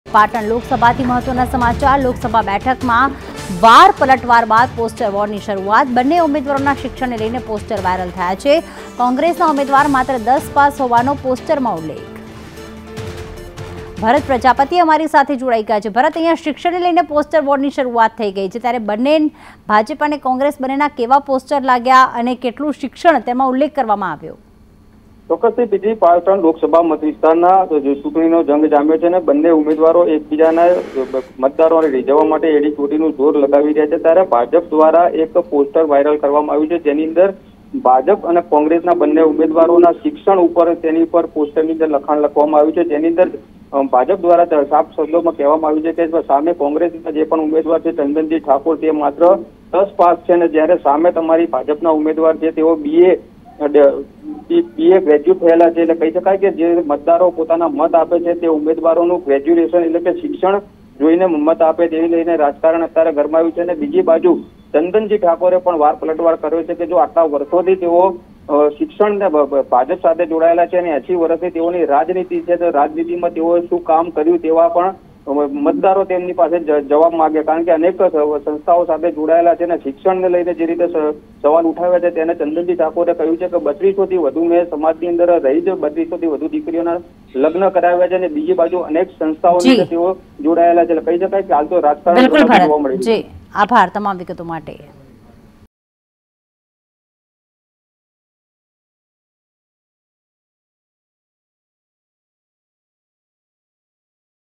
Lok Sabha Election | પાટણ લોકસભા ચૂંટણીમાં જામ્યો ચૂંટણી જંગ | Patan | Gujarat
પાટણ લોકસભા ચૂંટણીમાં જામ્યો ચૂંટણી જંગ
લોકસભા બેઠકમાં વાર પલટવાર બાદ પોસ્ટર વોર
બંને ઉમેદવારોના શિક્ષણને લઈ પોસ્ટર વાયરલ
કોંગ્રેસ ઉમેદવાર માત્ર 10 પાસ હોવાનો પોસ્ટરમાં ઉલ્લેખ
ભાજપ ઉમેદવાર BA ( ARTS) સાથે ગ્રેજ્યુએટ છે
શિક્ષણને લઈ પોસ્ટર વોર શરૂ થતા રાજકીય ગરમાવો